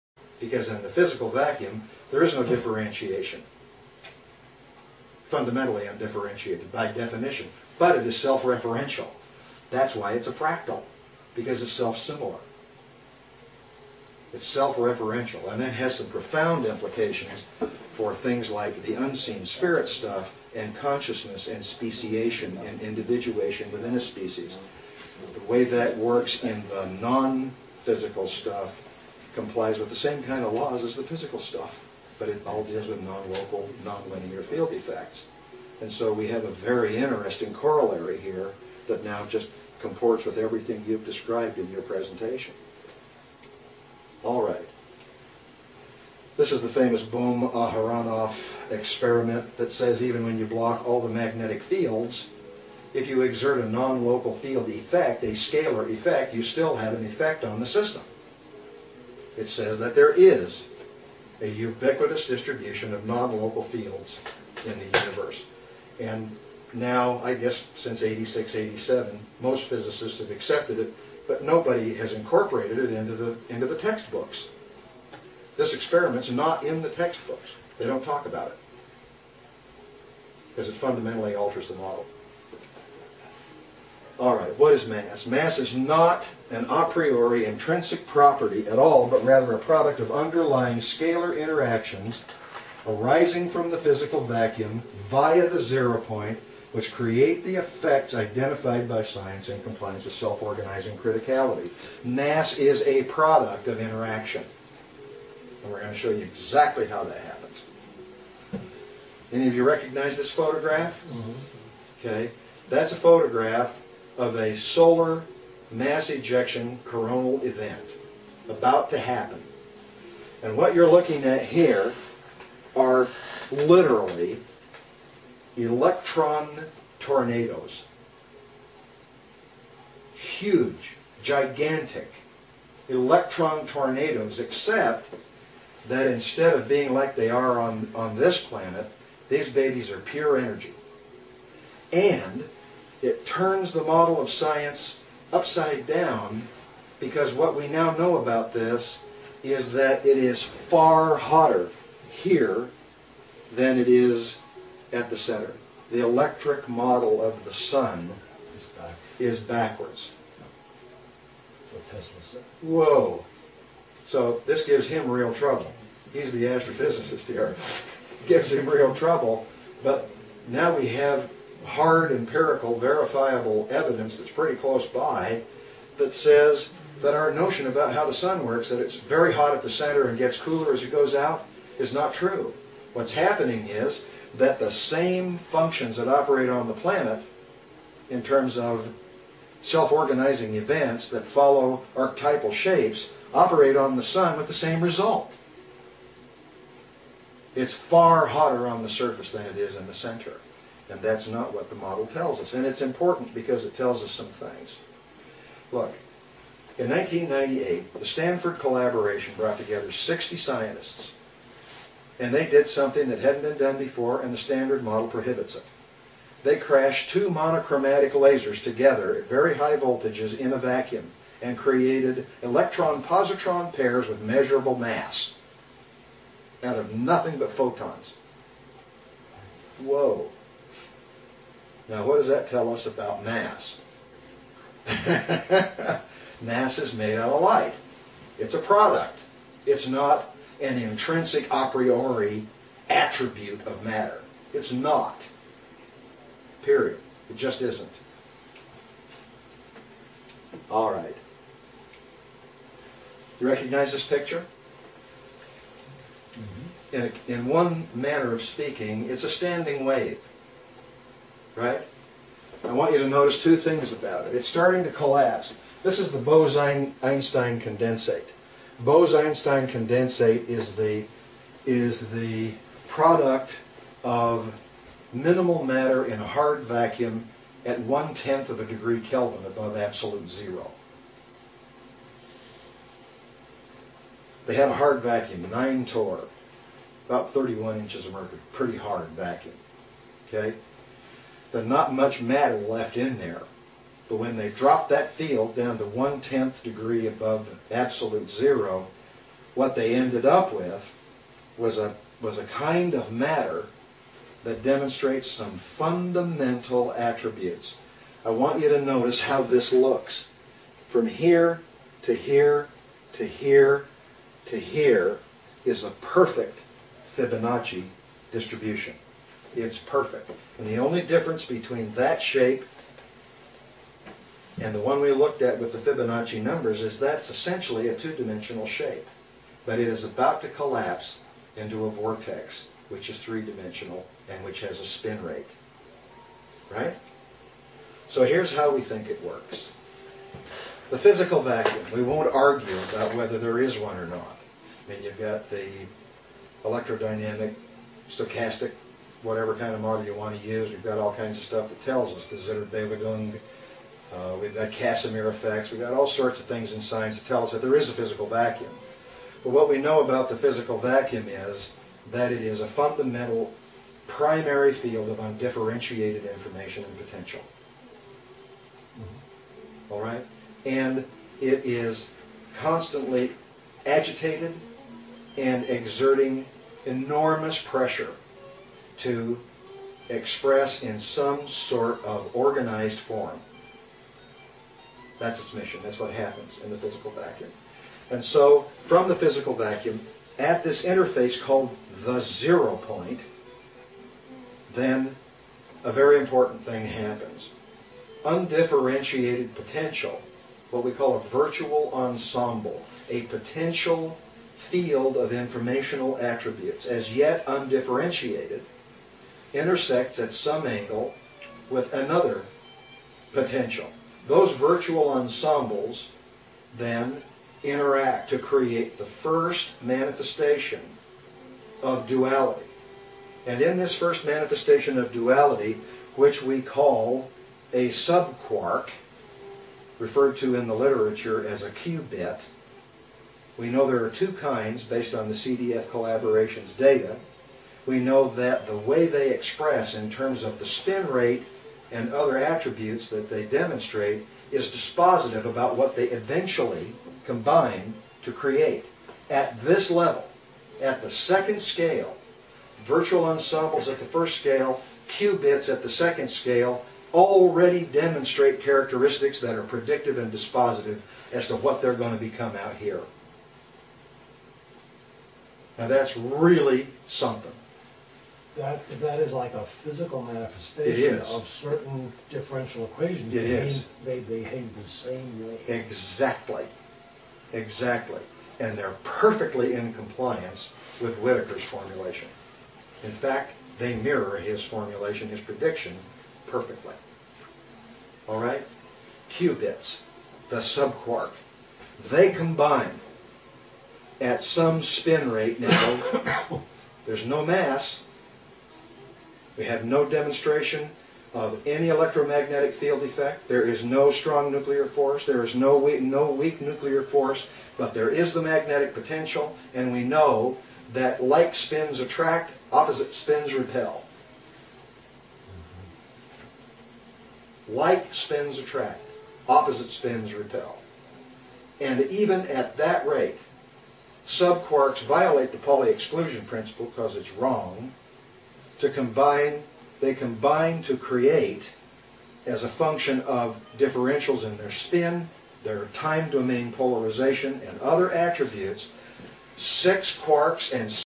Physics of Zero Point Energy by David Yurth Part 5/8
One of the most technical presentations on the Zero Point Energy and its relationship to convensional physics. This was one of many great presentations at Stephen Greer's AERO, LLC on August 20th, 2007.